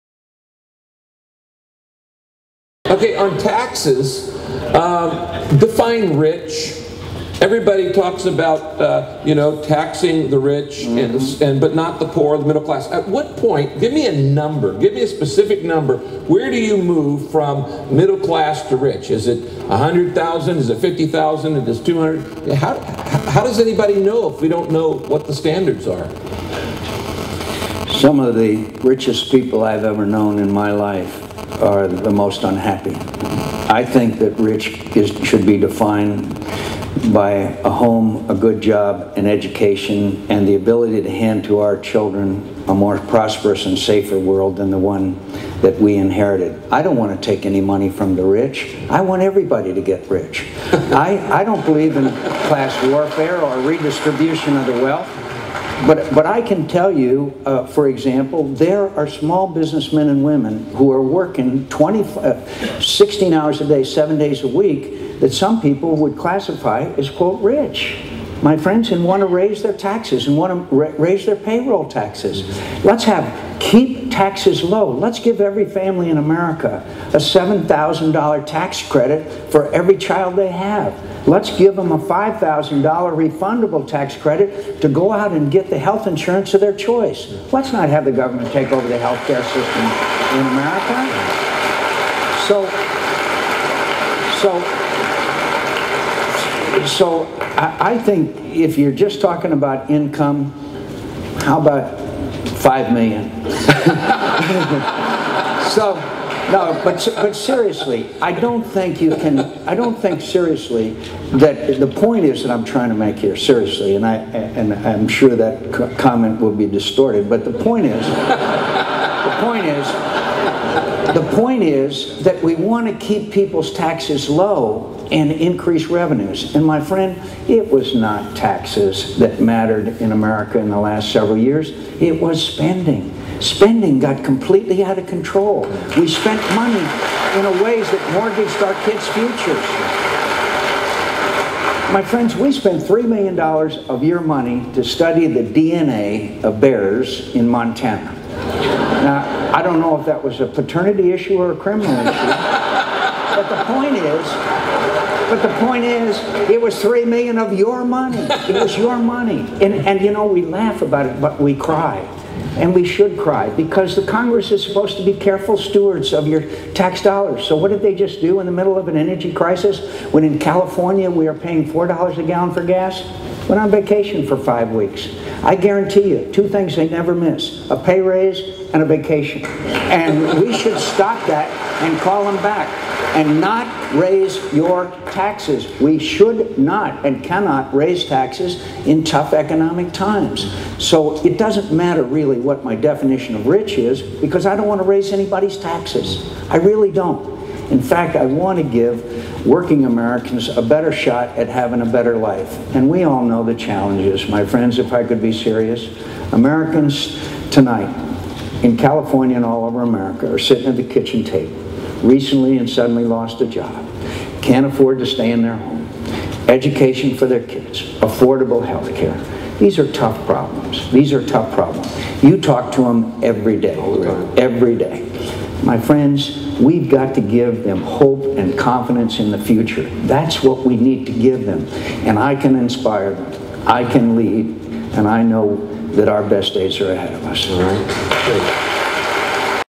John McCain: Saddleback Forum 08/16/08
John McCain
Saddleback Civil Forum
Lake Forest, CA 08/16/08

TRANSCRIPT:
RICK WARREN: "On taxes, define rich.  Everybody talks about, you know, taxing the rich, but not the poor or the middle class.  At what point, give me a number, a specific number.  Where do you move from middle class to rich, is it 100,000, is it 50,000, is it 200?  How does anybody know if we don't know what the standards are?

JOHN MCCAIN: Some of the richest people I've known in my life are the most unhappy.  I think that rich should be defined by a home, a good job, and an education, and the ability to hand to our children a more prosperous and safer world than the one that we inherited.  I don't want to take any money from the rich.  I want everybody to get rich.  I don't believe in class warfare or the redistribution of the wealth.  But I can tell you that for example, there are small business men and women who are 20, 16 hours a day, 7 days a week, that some people would classify as quote rich.  My friends, and want to raise their taxes, and want to raise their payroll taxes. Lets have, keep taxes low, give every family in America a 7,000 dollar tax credit for every child they have.  Lets give them a $5,000 refundable tax credit to go out and get the heath insurance of their choice.  Lets not have the government take over the health care system in America.  So, so, so I think if you're just talking about income, How about 5 million?  So, no but seriously, I don't think you can, I don't think seriously, that the point is I'm trying to make here seriously, and I'm sure that comment will be distorted.  But the point is, the point is that we want to keep people's taxes low and increase revenues.  And my friends, it was not taxes that mattered in America in the last several years.  It was spending, spending got completely out of control.  ...] We should not and cannot raise taxes in tough economic times.  So it really doesn't matter what the definition of rich is because I don't want to raise anyone's taxes, I really don't. [...]"

LABEL: JM CA 08-16 (KF#8) CLMPX - ClipD